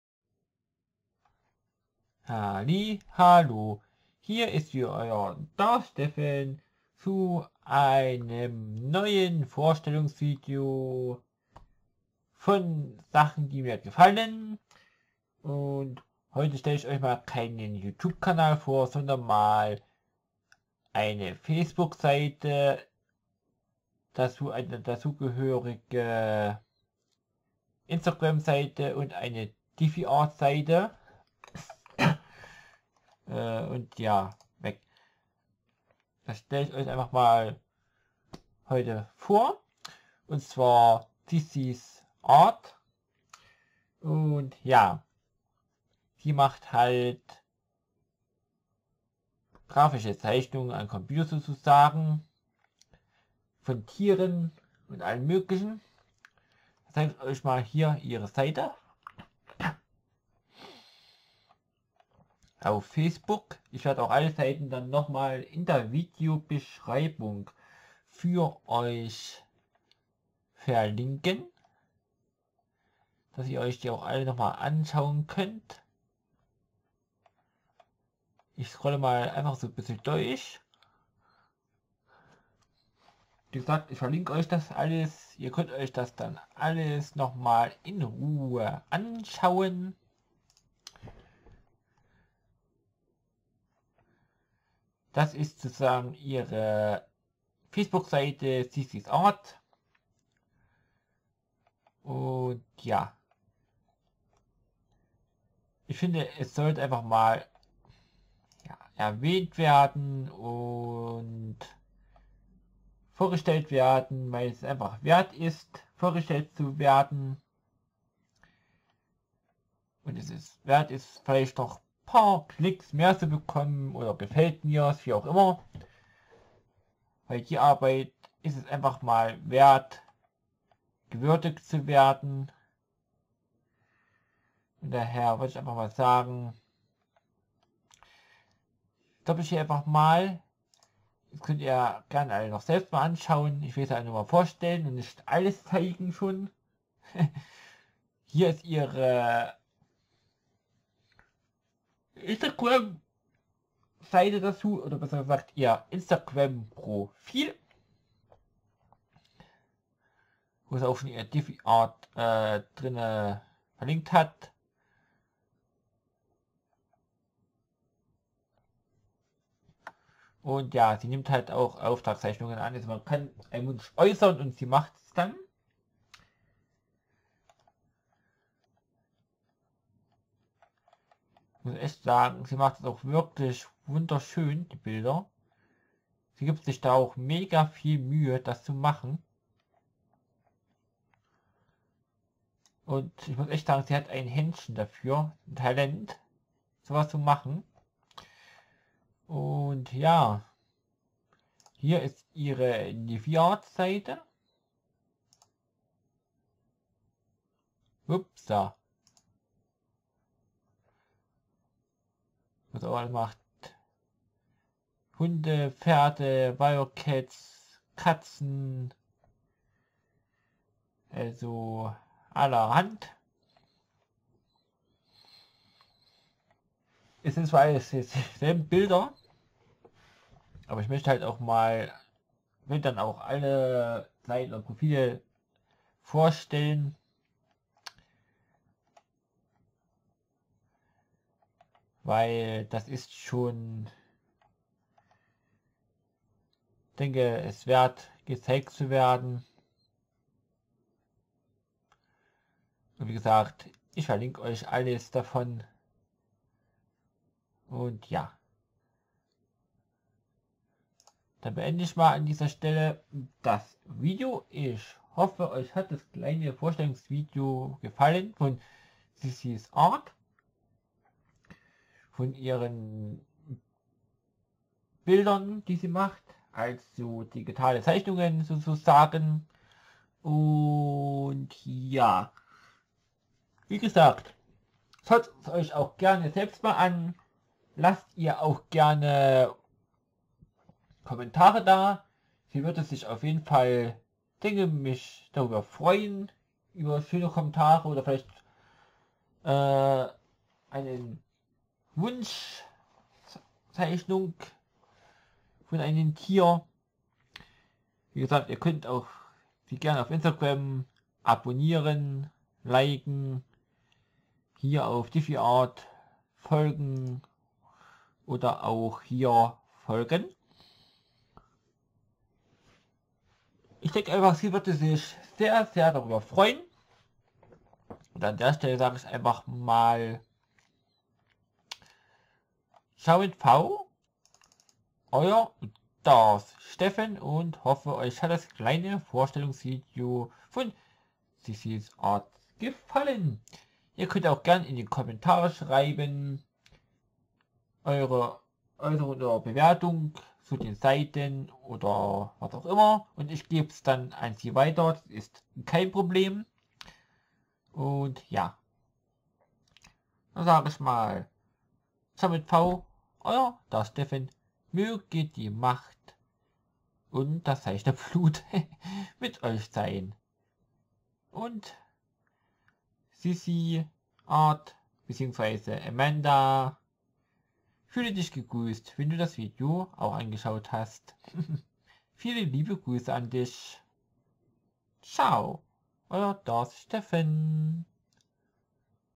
Vorstellung einer Grafischen Zeichnerin(Künstlerin) Sissi's ART
Hier ein weiteres Vorstellungs Video
einer Guten Freundin von dir.
Und weill die Zeichnungen die sie
m,acht einfach weiter zu Empfehlen
sind und Sie das echt Super macht
lasst gerne euer Feedback da und
auch gerne eure Meinungen Sie würde
sich denke sehr Darüber Freuen.

Vielen Dank euer Darth Steffen

Hier die Links zu den Seiten